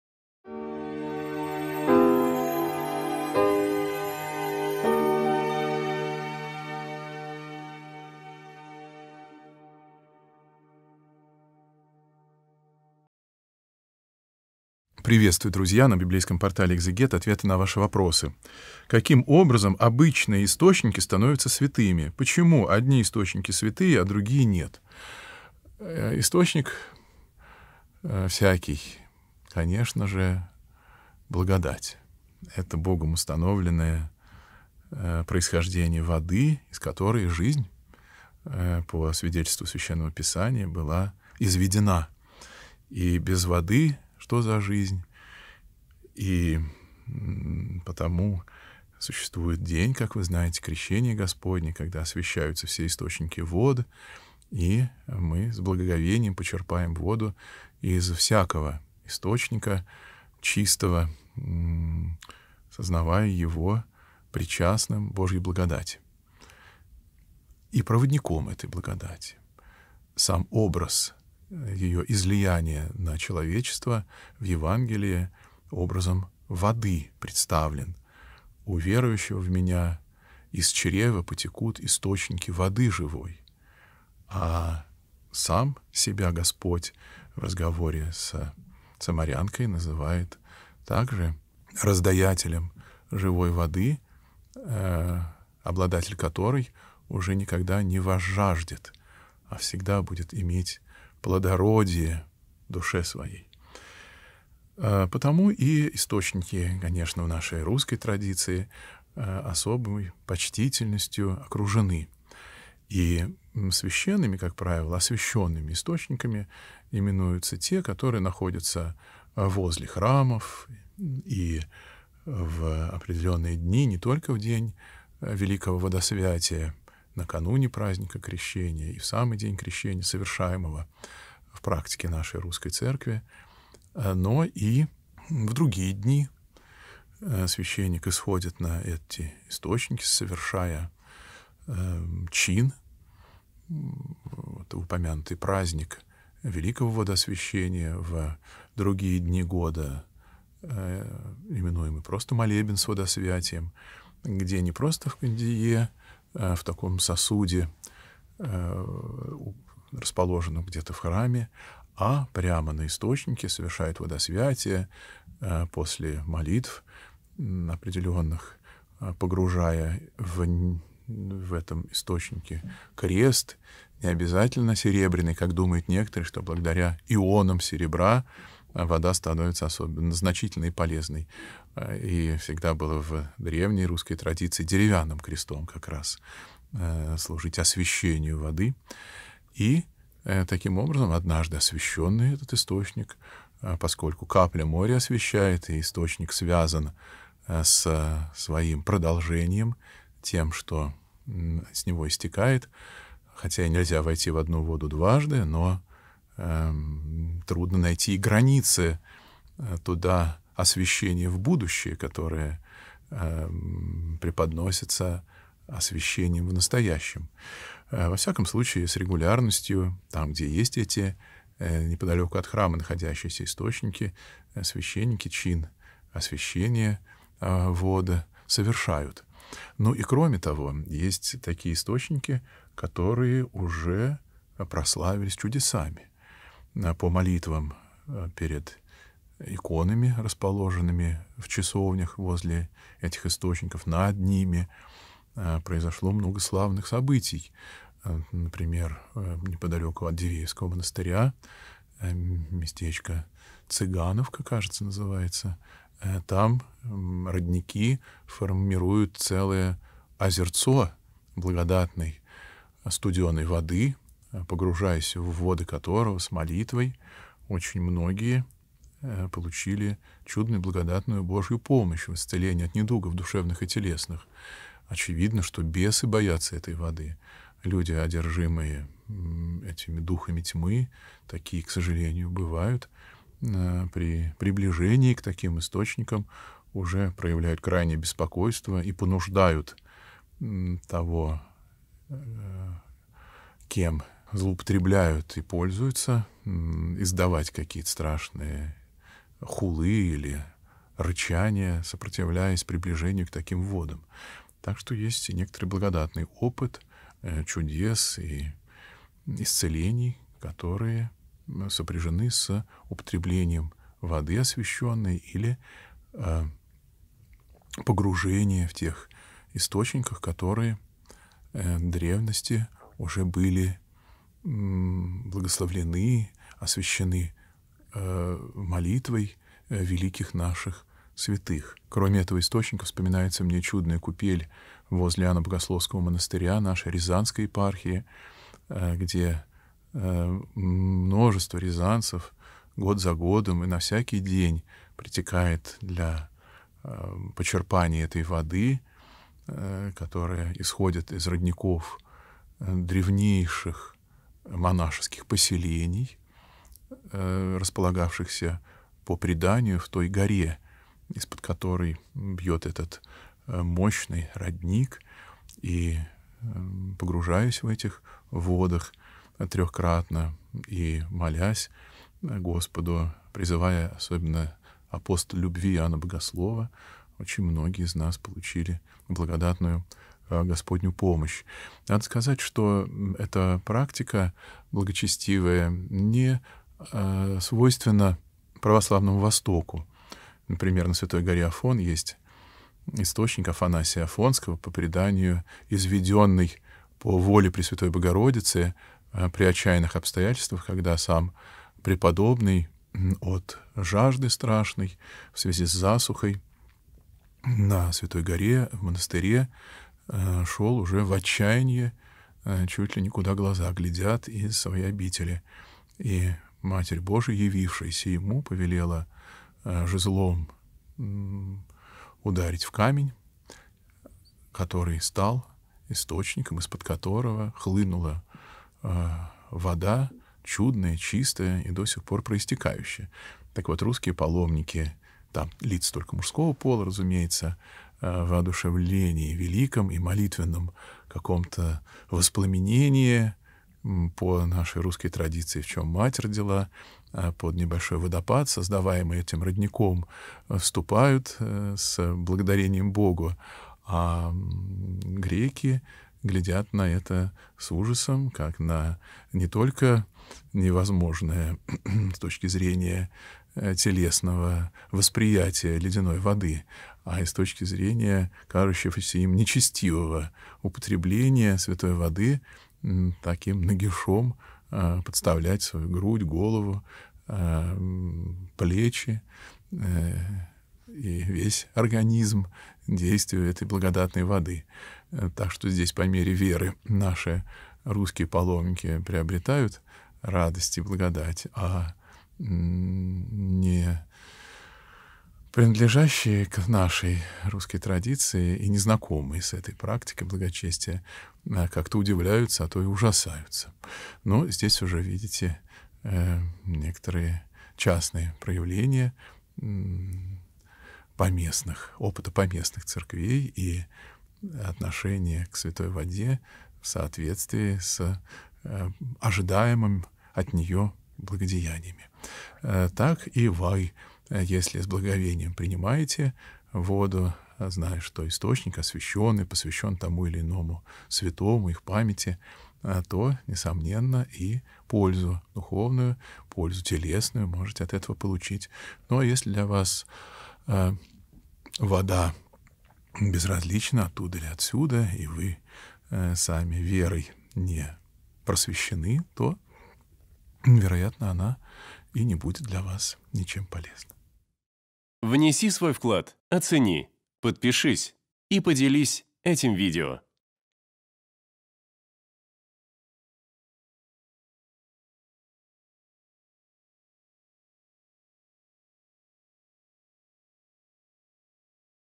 Почему одни источники являются святыми, а другие нет ? Игумен Лука (Степанов)
В сегодняшнем видео с игуменом Лукой (Степановым) отвечаем на вопрос: "Почему одни источники являются святыми, а другие нет?".  Касаемся следующих тем:

🔸 Источник благодати.
🔸 Живая вода.
🔸 Водосвятие.
🔸 Исцеление недугов.
🔸 Источник Афанасия Афонского.